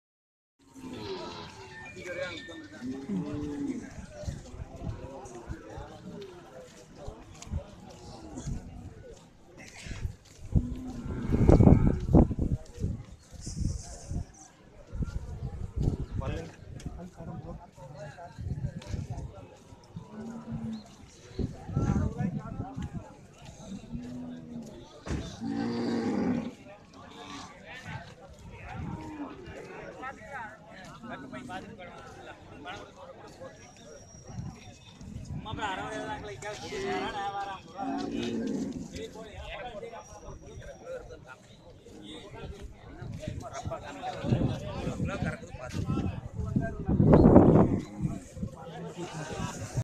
பழையகோட்டை மாட்டு தாவணி 03/06/2018
பழையகோட்டை மாட்டு தாவணி விறுவிறுப்பாக அதிக நல்ல காங்கேயம் மாடுகளின் வரவோடு தொடங்கியது. நல்ல வர்கம் காங்கேயம் மாடு வாங்க ஞாயிறு தோறும் நடக்கும் பழையகோட்டை மாட்டுத்தாவணிக்கு வாரீர் !!